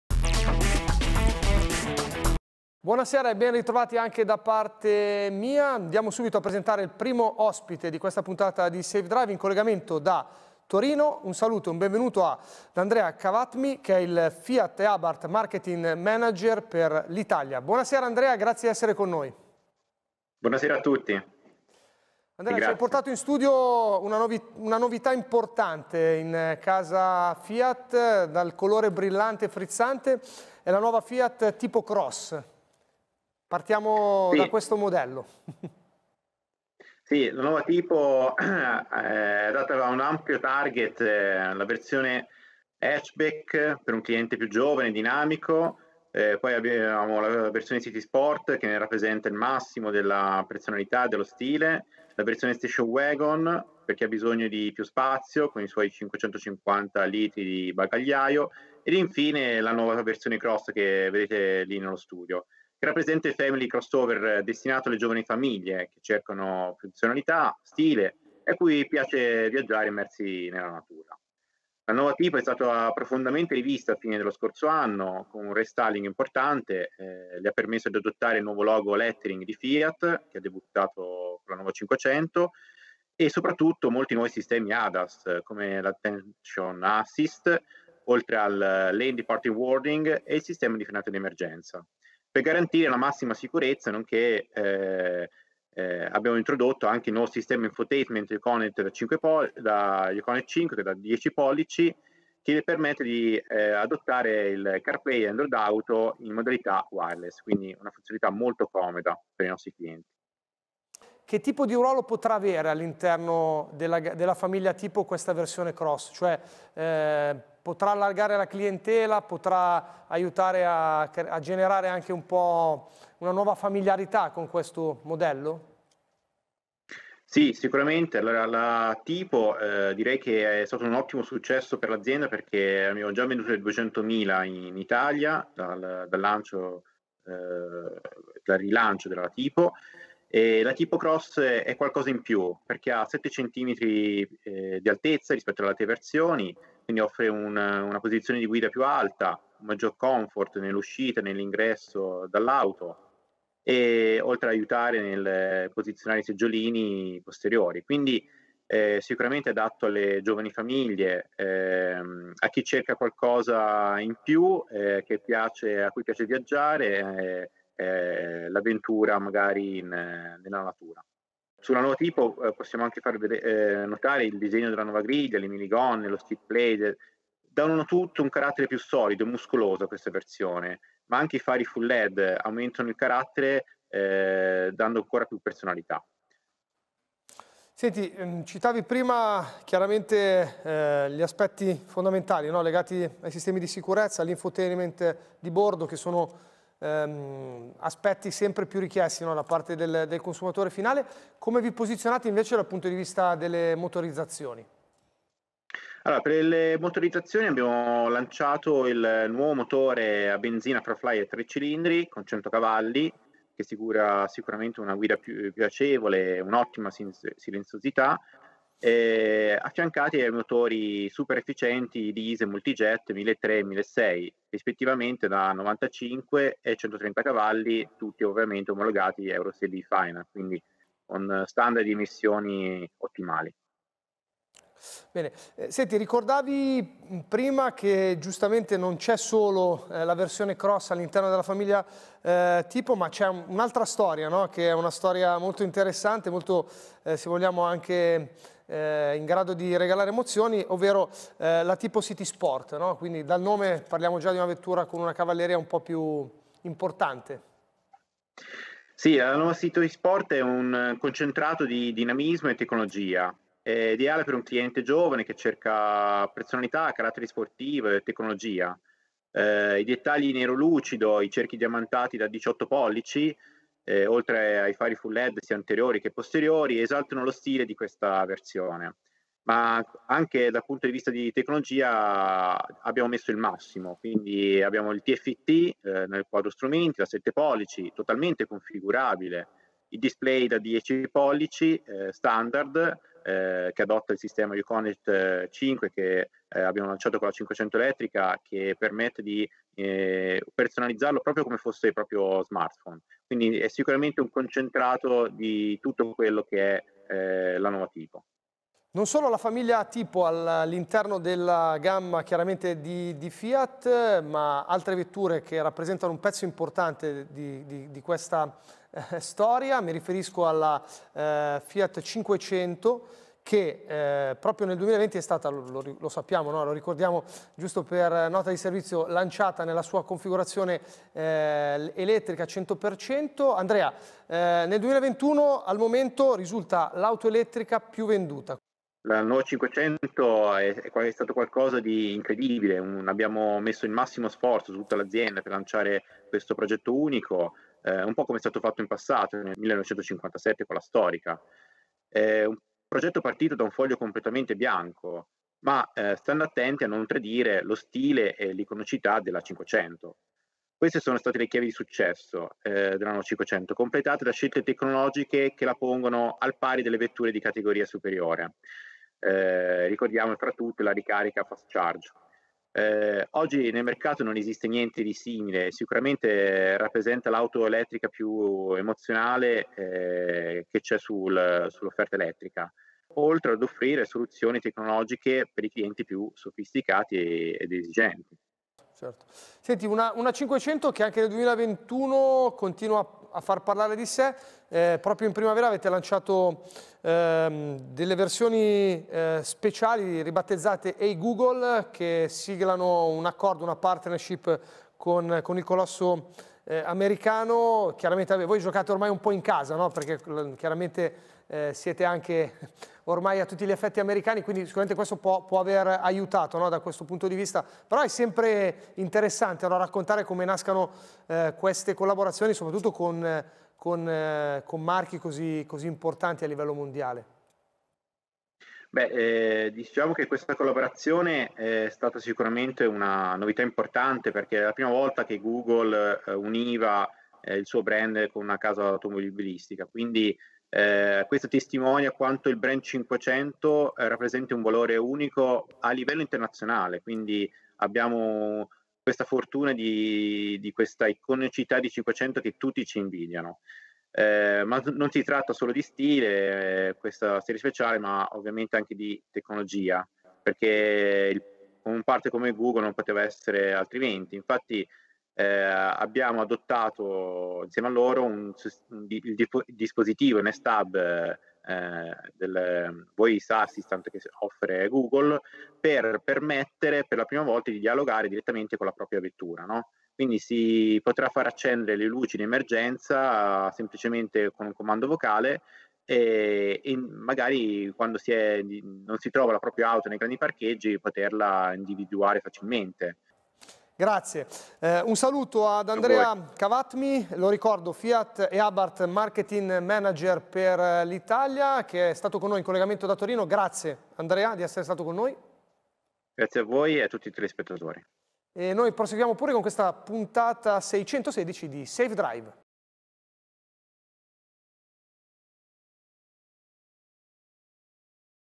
In collegamento: Andrea Khawatmi - Senior Brand Manager Fiat & Abarth Mercato Italia
Negli studi di Safe-Drive fa il suo ingresso per la prima volta Fiat Tipo Cross. Una novità che ha esordito alla fine del 2020.  Siamo andati alla scoperta del ruolo di questo modello all’interno della gamma e  delle sue principali caratteristiche. Ne è uscito un crossover dinamico che si rivolge alle giovani  famiglie. E’ stato rivolto uno sguardo anche verso la componente più “ribelle” della famiglia Tipo, ovvero la Tipo City Sport nonché la versione full Electric di Nuova 500.